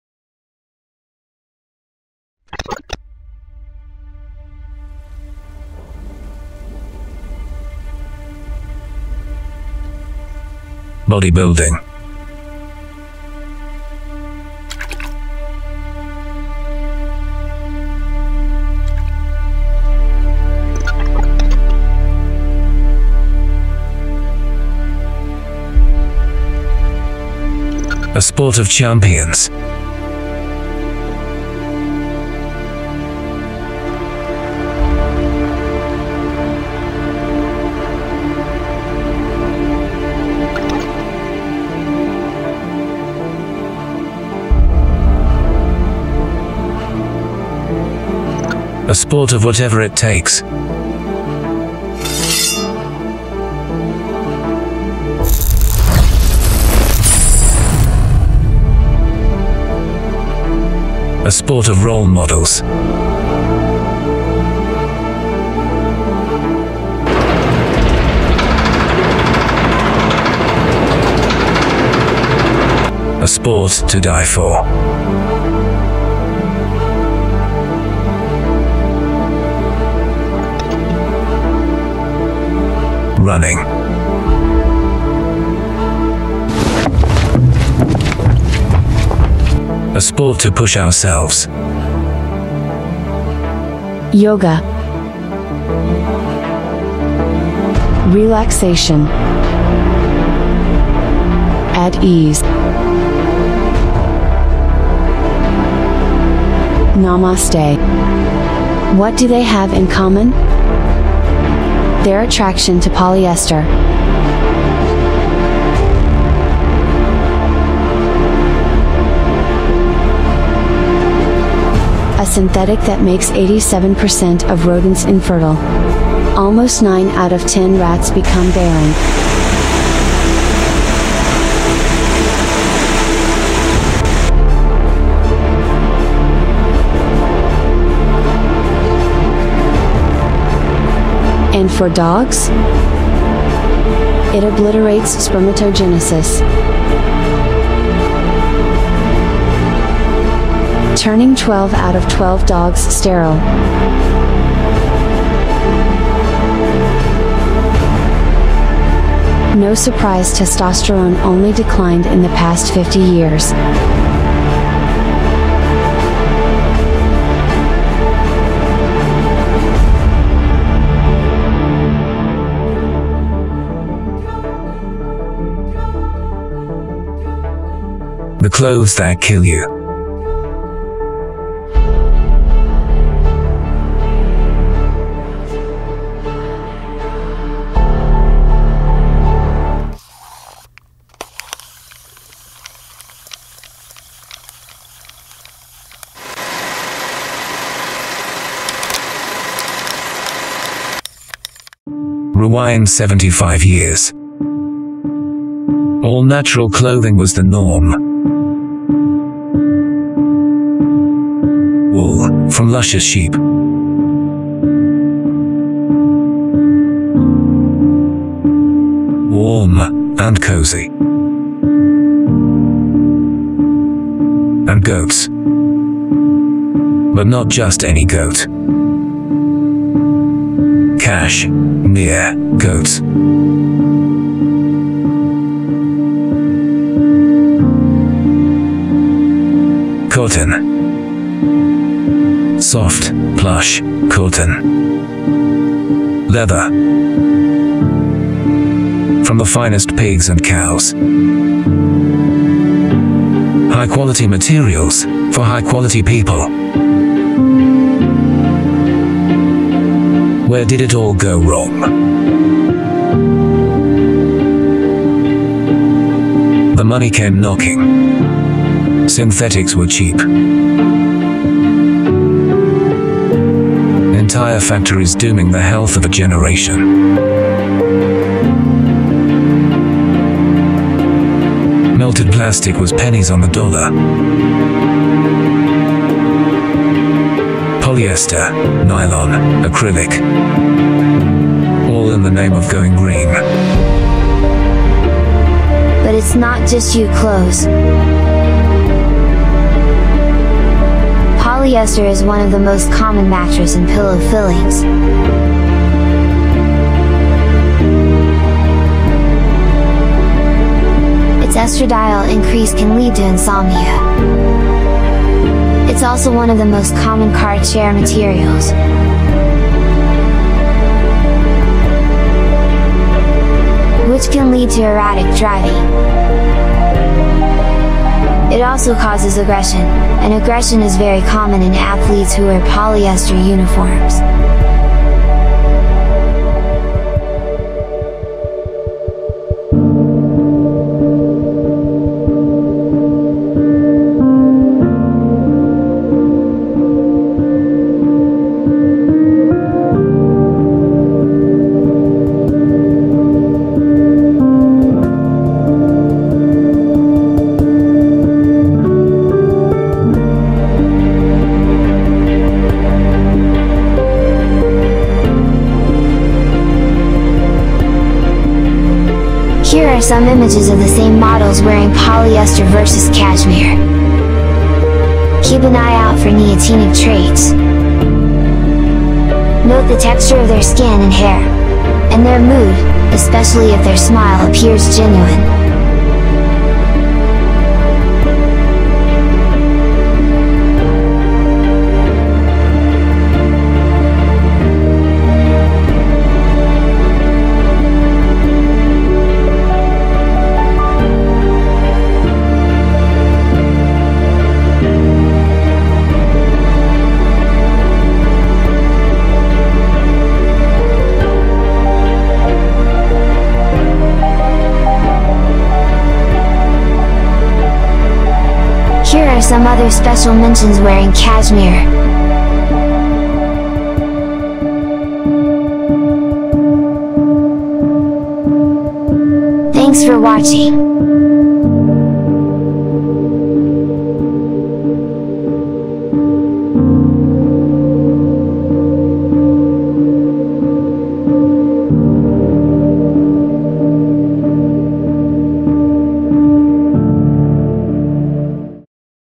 The Clothes that Kill You - A documentary on the effects of Clothes on Hormones
A brief documentary on the effects of different clothing fabrics on hormones .

***RE-UPLOAD due to audio issues

It really helps keep me going